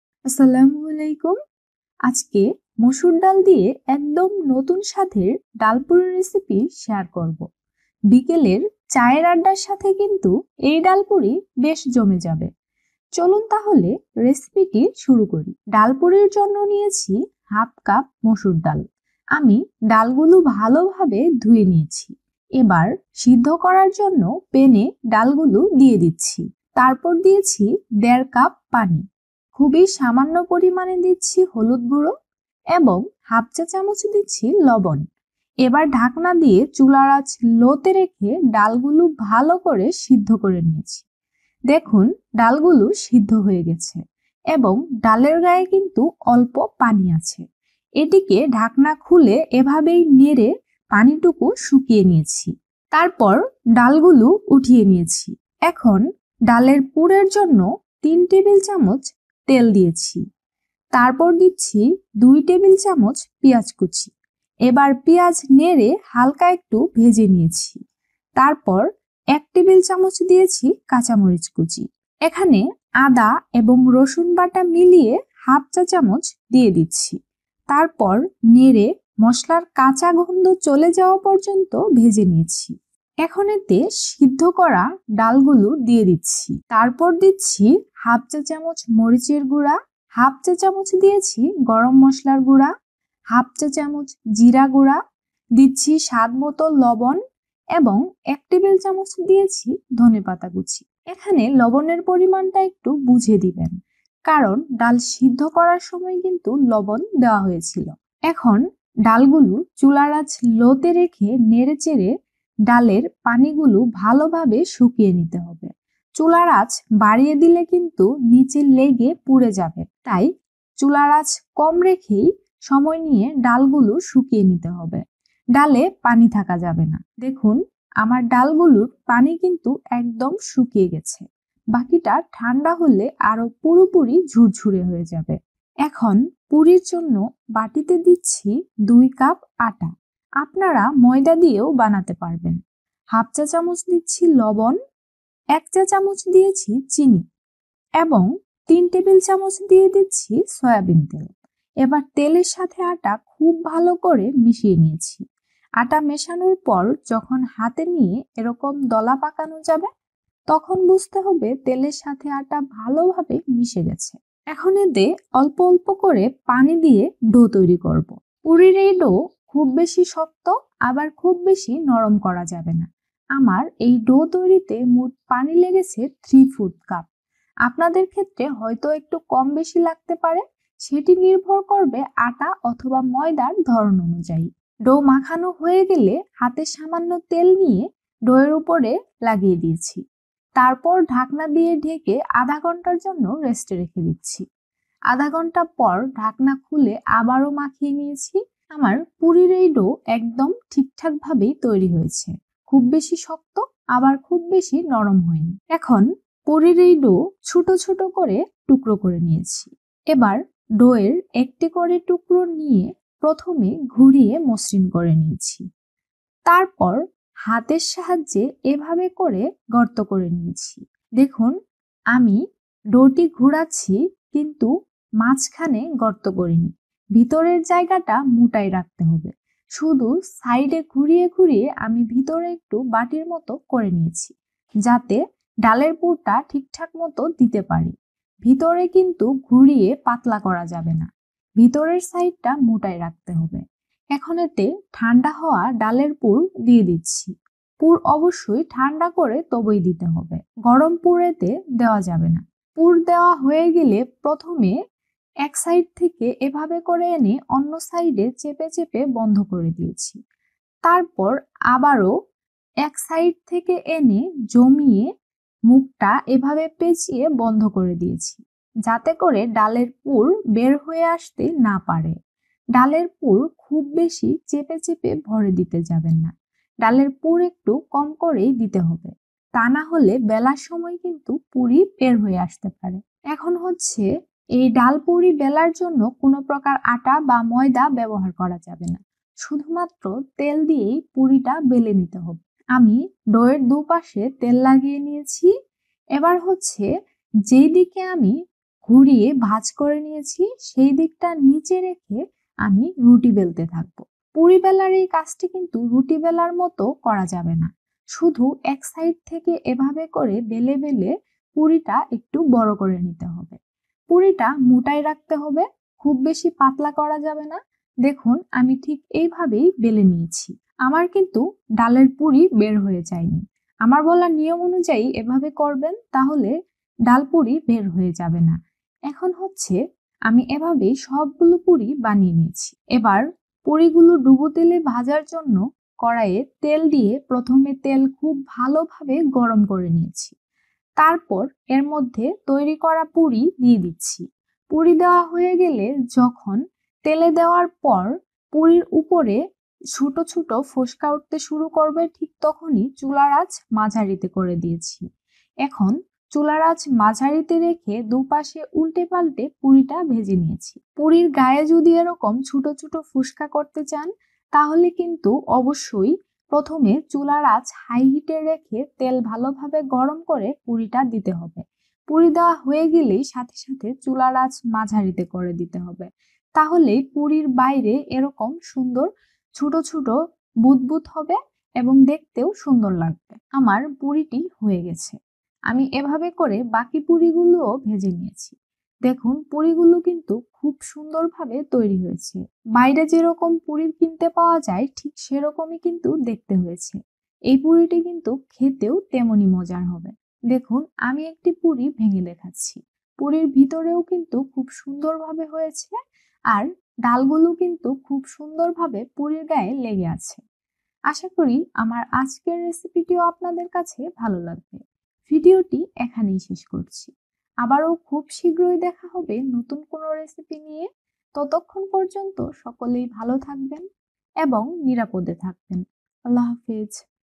একদম নতুন অপূর্ব স্বাদের খাস্তা ডালপুরি রেসিপি | হোটেলের মতো বানাবার কৌশলসহ| dal puri recipe bengali
একদম নতুন ধরনের অপূর্ব স্বাদের মুসুর ডালের ডাল পুরি রেসিপি, একবার খেলে বারবার খাবেন।
হোটেলের মতো বানাবার এবং ডালের পুর বের না হবার কৌশলসহ।

আর হ্যাঁ, আশা করি রেসিপিটি আপনি অবশ্যই একবার বাসায় তৈরি করবেন এবং কেমন হয়েছে তা আমাকে জানাতে ভুলবেন না, আপনার মূল্যবান মতামত আমাকে অনেক বেশি উৎসাহ দেয় নতুন ভিডিও তৈরিতে, ধন্যবাদ।